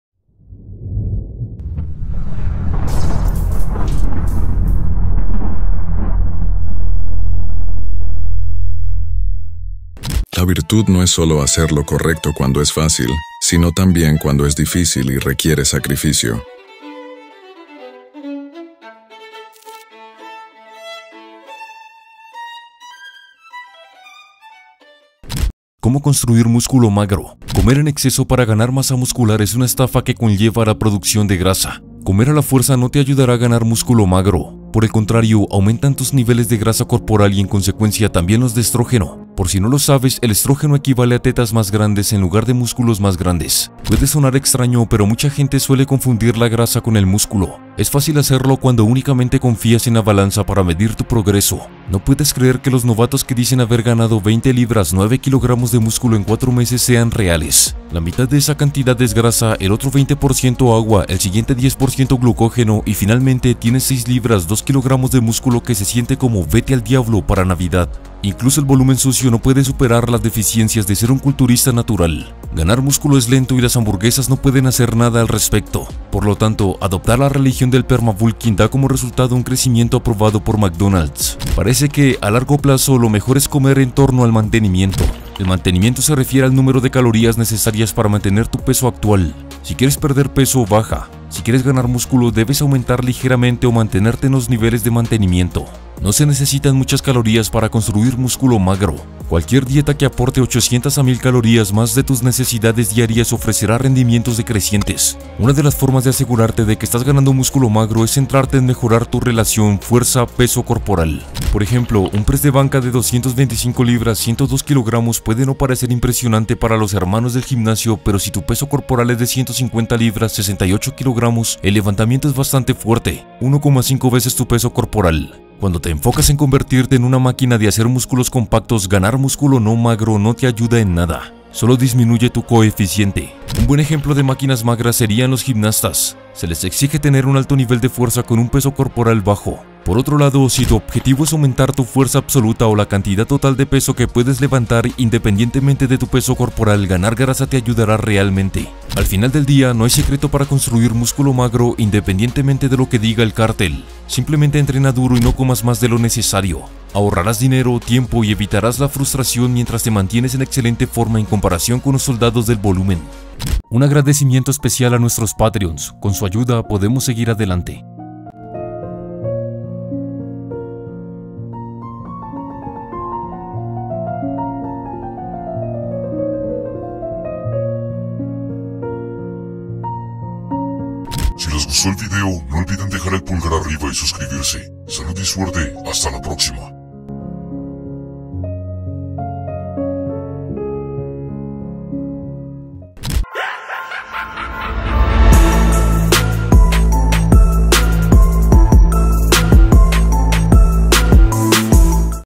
Cómo Construir MÚSCULO 'Magro
Comer en exceso para ganar masa muscular es una estafa que conlleva a la producción de grasa. Comer a la fuerza no te ayudará a ganar "músculo magro". Por el contrario, aumentan tus niveles de grasa corporal y, en consecuencia, también los de estrógeno. Por si no lo sabes, el estrógeno equivale a tetas más grandes en lugar de músculos más grandes.